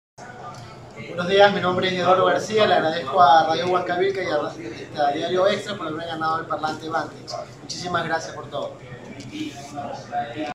Entrega del parlante Vantec, gracias a Radio Huancavilca y Diario Extra
Eduardo García, participó y ganó en Gol Ecuador, de lunes a viernes 14:30 - 16:30.